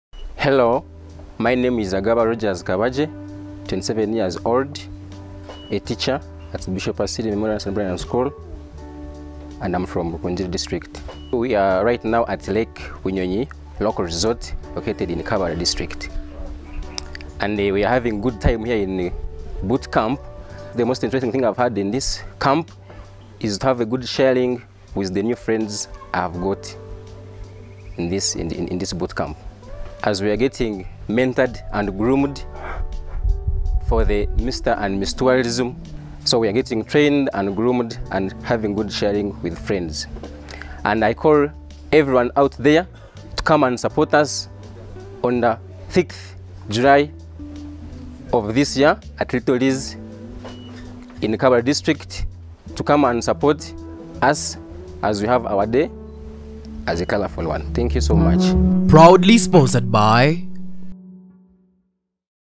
Miss & Mr Tourism Kigezi - Agaba Rogers Kabagye
This is Agaba Rogers Kabagye, 27 years of age, from Rukungiri, a Miss & Mr Tourism Kigezi contestant.

The 9th Miss & 3rd Mr. Tourism Kigezi Grand Finale and Crowning Live stream. Happening at Little Ritz Gardens Kabale will feature Performing Artists including  Allan Tonix, Fact Zamani, and Sir Kisoro, among many others.